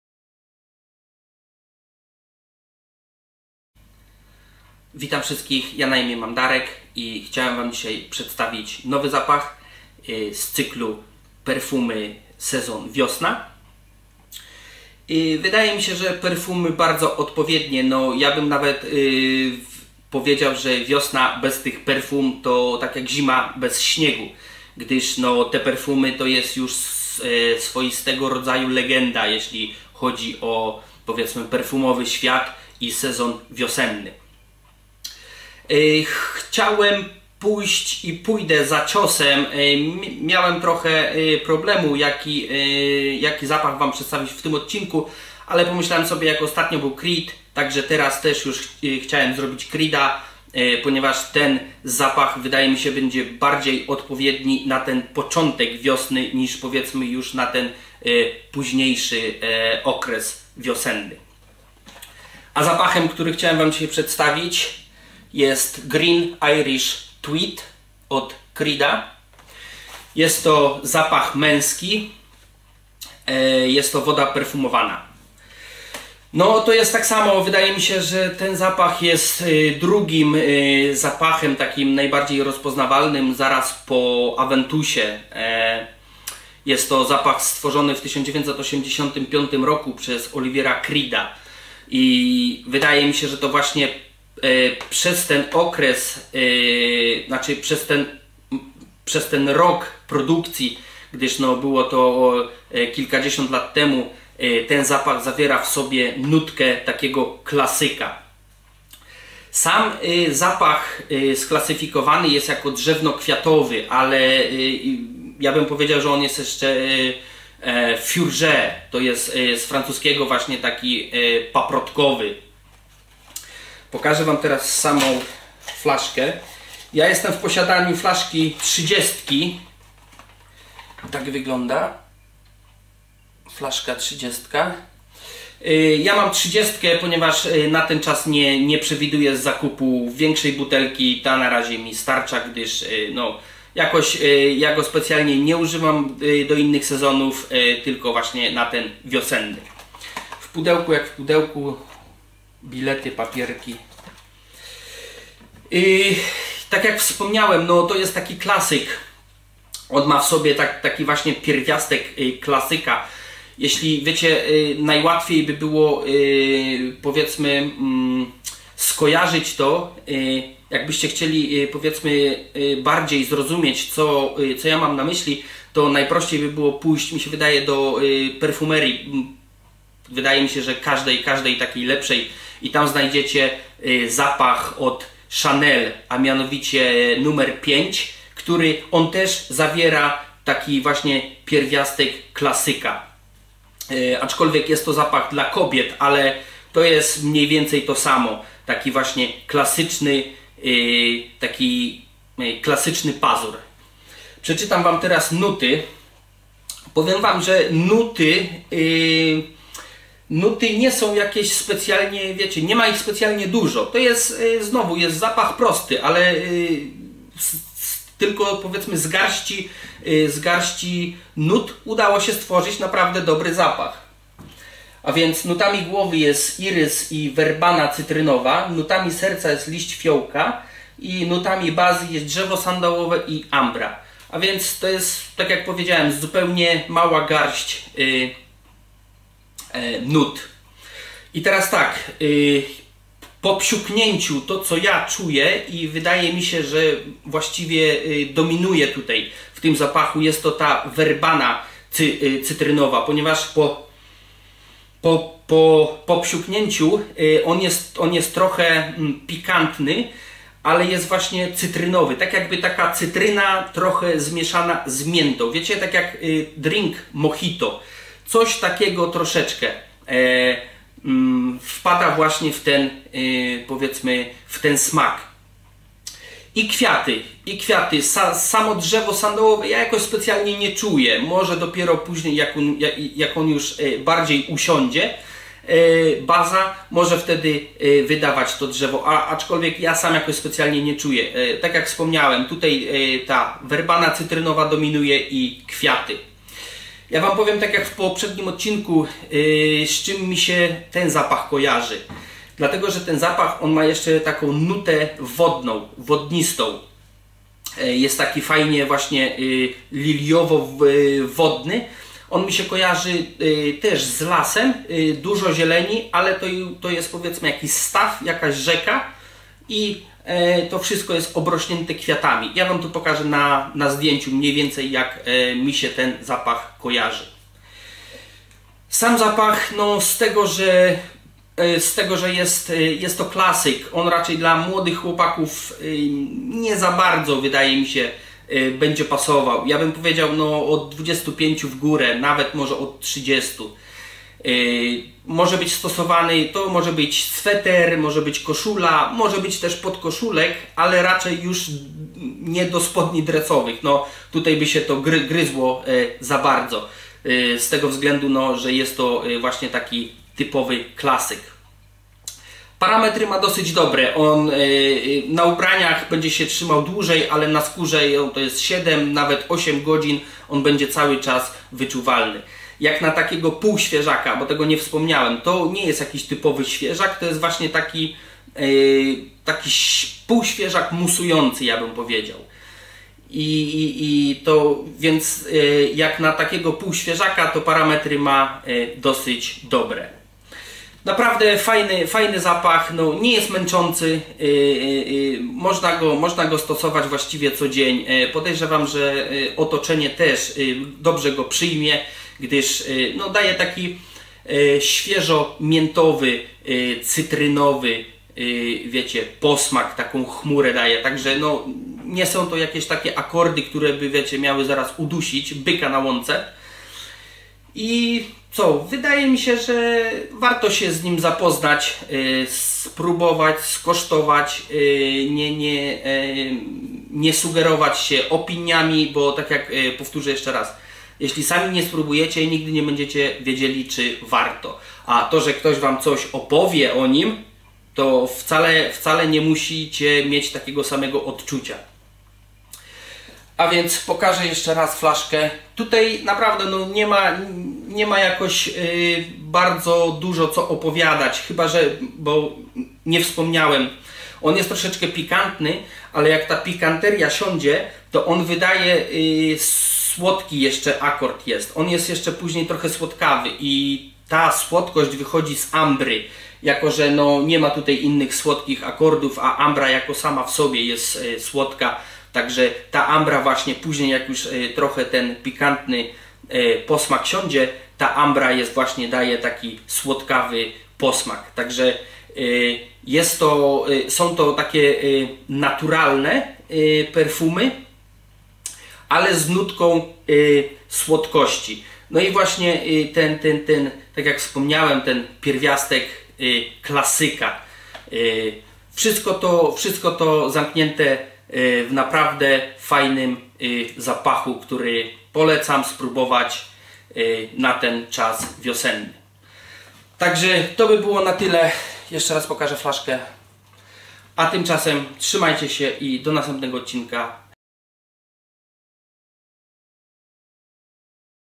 Perfumy: Sezon Wiosna - Creed "Green Irish Tweed" EDP
Zainspirowany kultowym dżentelmenem Green Irish Tweed jest zarówno świeży, jak i drzewny. Przywołując bujną zieleń Szmaragdowej Wyspy, świeżo ściętą trawę i świeżą morską bryzę, ten klasyczny męski zapach łączy cytrynę, indyjską werbenę i orzeźwiającą miętę z zielonymi nutami fioletowych liści. Utrzymujący się florencki irys, drzewo sandałowe i ambra uzupełniają ten orzeźwiający i ponadczasowy zapach.